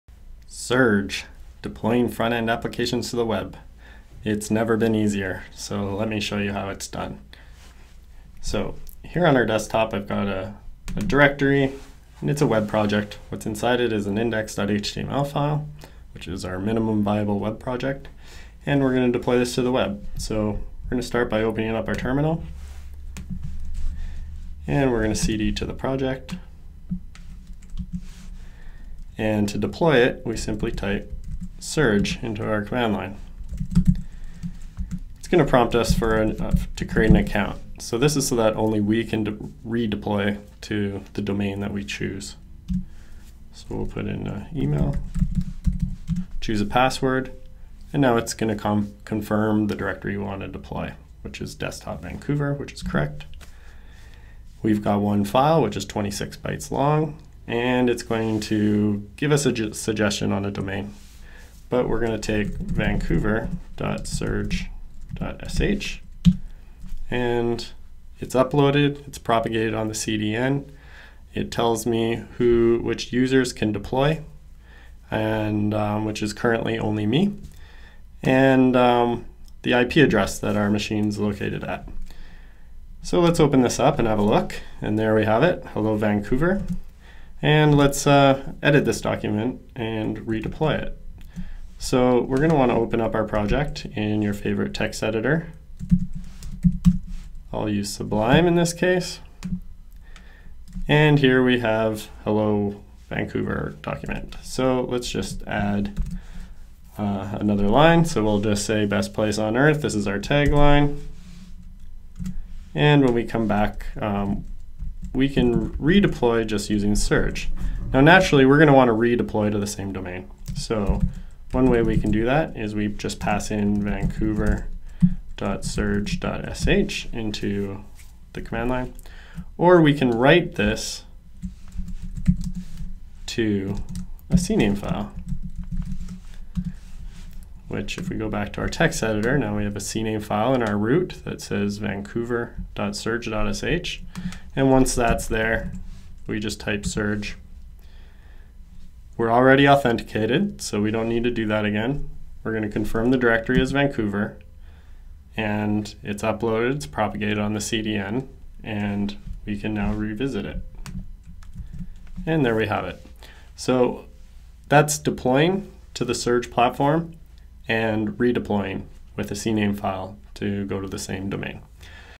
Introducing Surge
Quick demonstration on how to publish to the Surge CDN and re-publish to the same domain.

Install by running...
`npm install -g surge`